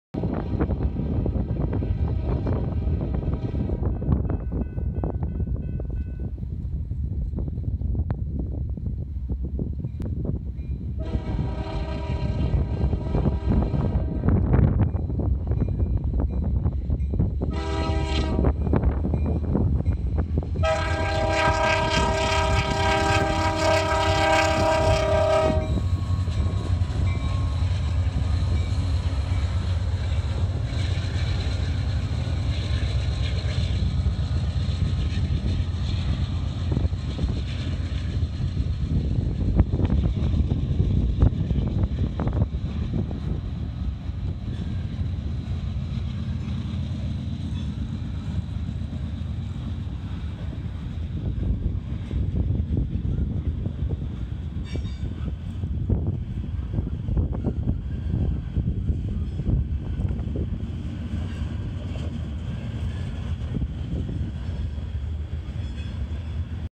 CN Freight Train in South Fulton, TN
On 3/30/19 I caught this CN freight train near South Fulton. There are 2 CN units leading. Enjoy!